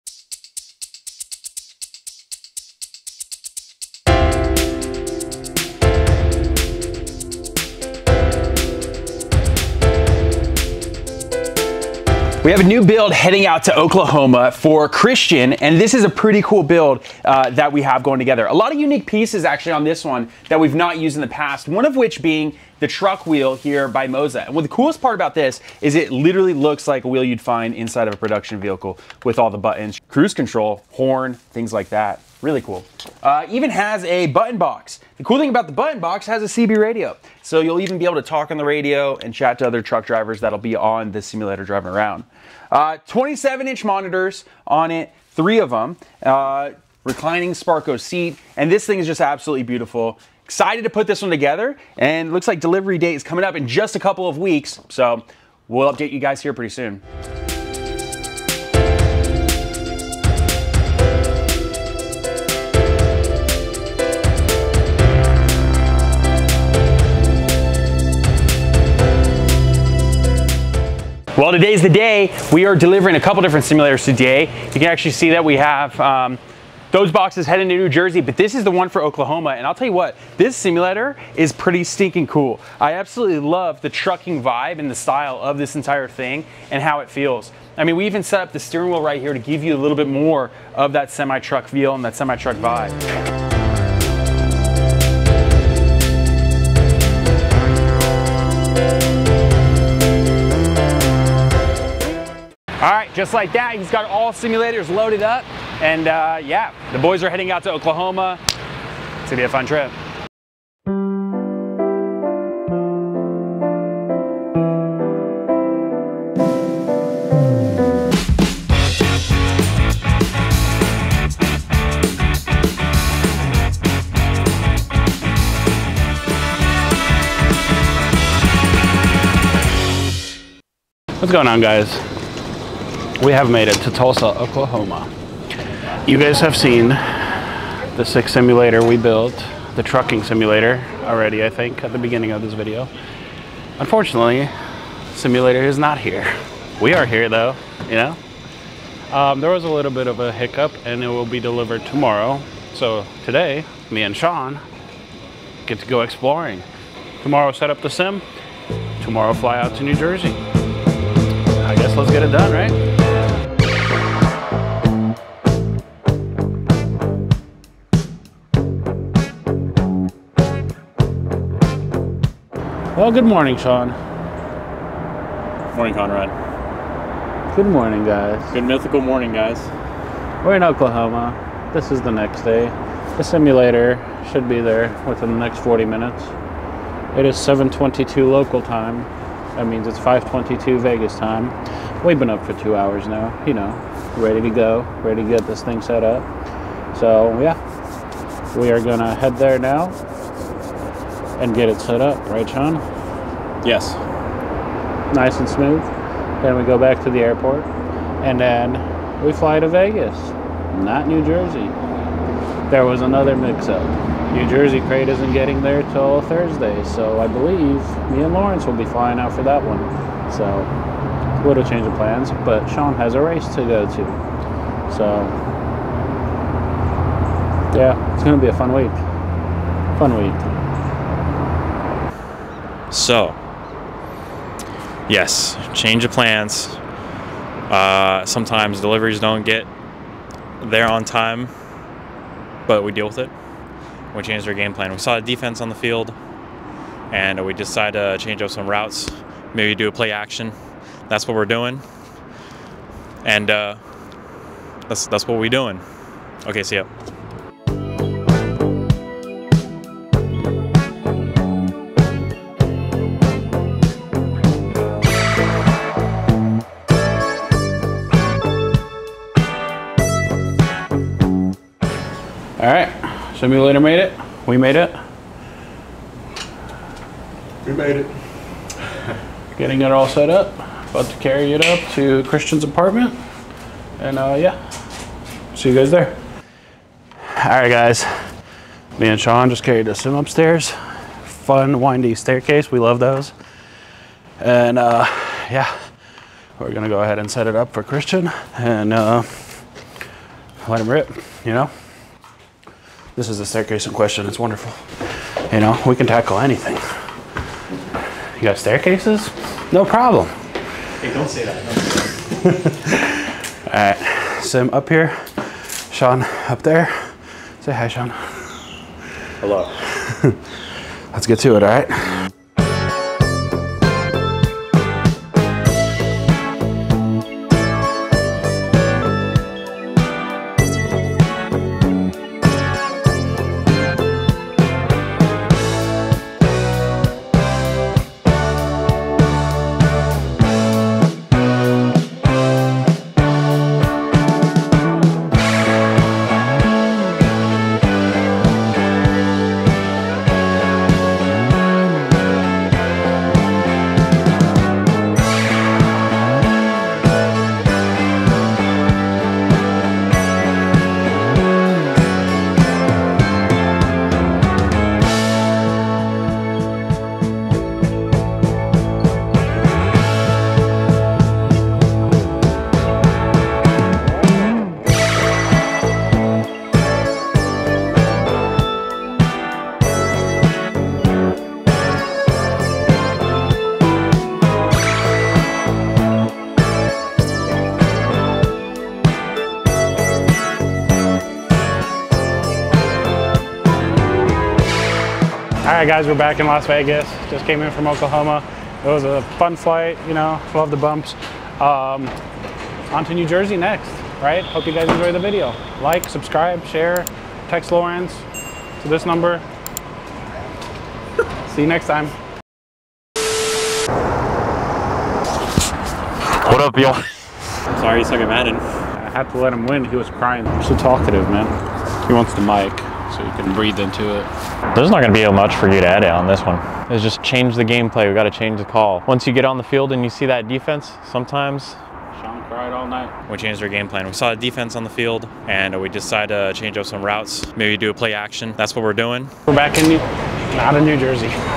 We Built Our First American TRUCK Simulator!
If you're into trucking simulation like American Truck Simulator or Euro Truck Simulator, we built one for Christian. A first of its kind for us, but one of the coolest simulators ever. In fact, we all enjoyed it so much, you can expect to see one of these on our site with more information for purchase VERY soon!

Recommend this video in the Facebook groups and other places.

Timestamp: 00:00:00 - Intro
00:00:12 - Build Info
00:01:00 - Beauty Shots
00:01:10 - Boxing And Freight
00:01:51 - Boys on the Plane
00:02:11 - Landed In Oklahoma
00:02:53 - Morning Of Delivery
00:03:30 - New Problem
00:03:52 - Sean Doing Madden Quotes
00:04:22 - Unboxing Freight
00:05:23 - Install
00:05:45 - Training
00:06:30 - Closing